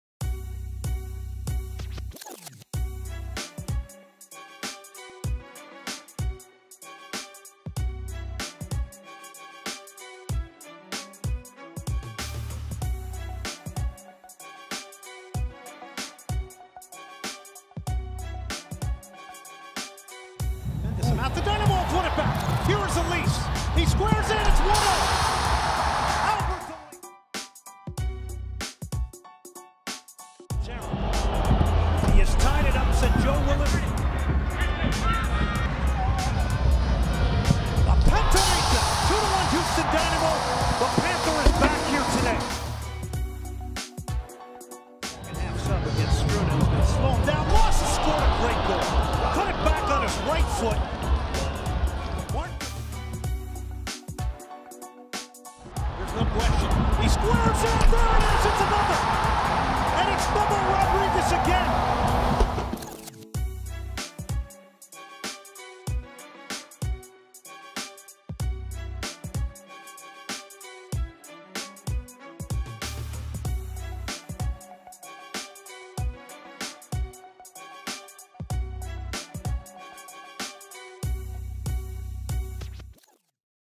Staying Hot at Home | Dynamo Rewind
Re-live the Houston Dynamo's 3-2 win over Vancouver Whitecaps FC on March 16 with Dynamo Rewind, presented by your Greater Houston Honda Dealers.